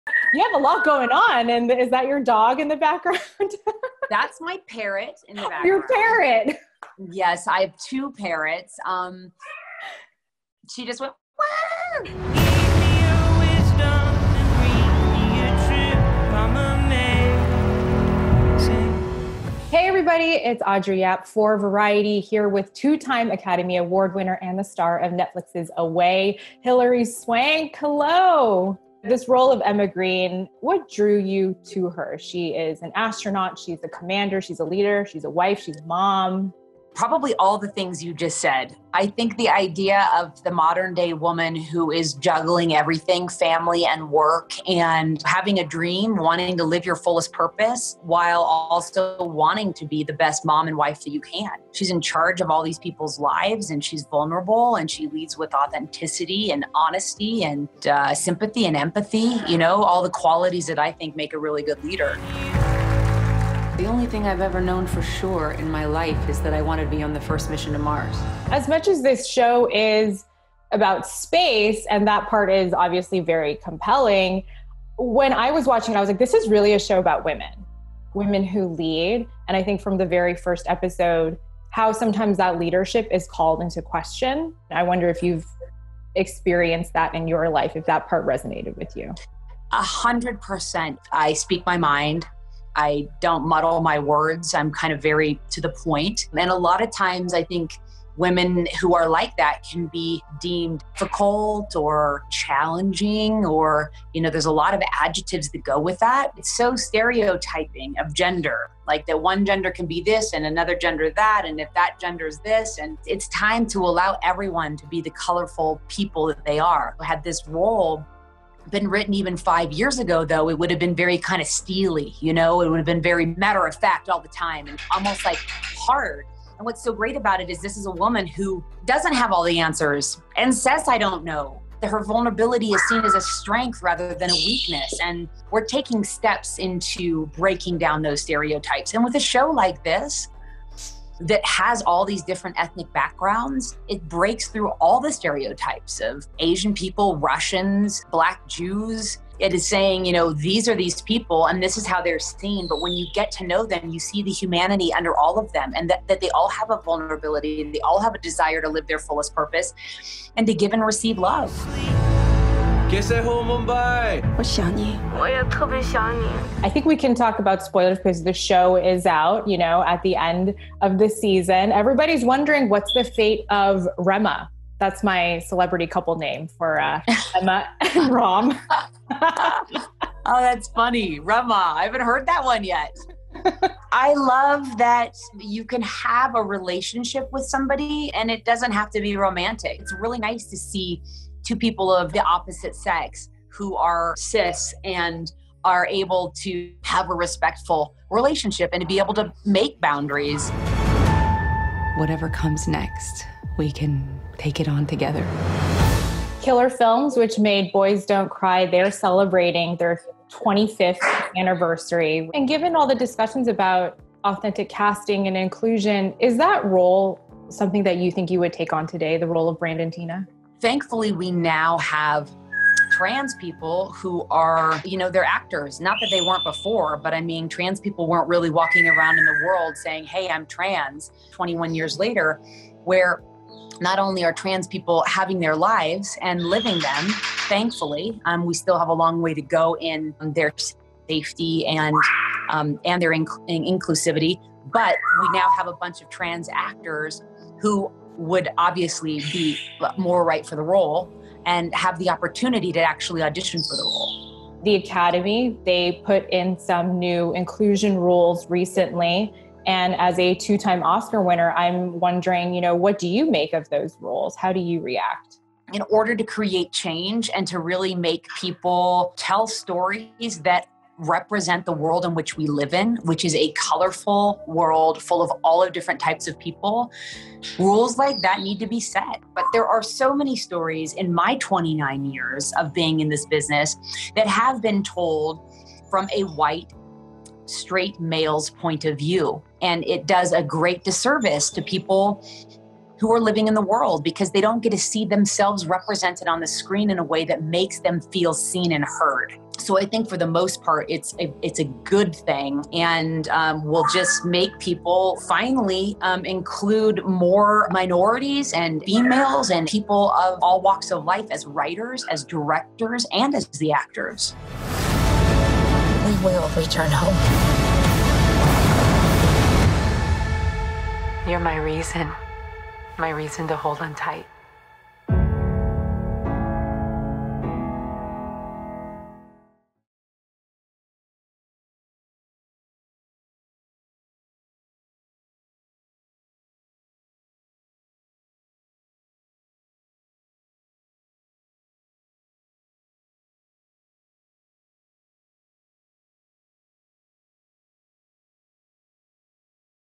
'Away' Star Hilary Swank on Oscars New Inclusion Rules and the Legacy of 'Boys Don't Cry'
Academy Award winner Hilary Swank sits down with Variety news anchor Audrey Cleo Yap to talk about her Netflix series "Away" and the future of Ram and Emma (SPOILERS).

Swank also looks back on the legacy of "Boys Don't Cry" and how far representation for trans people and actors have come. Swank won her first Oscar in 2000 for her portrayal of Brandon Teena, a young trans man, forced to live a life on the run as he tries to keep his gendered identity a secret from small town locals.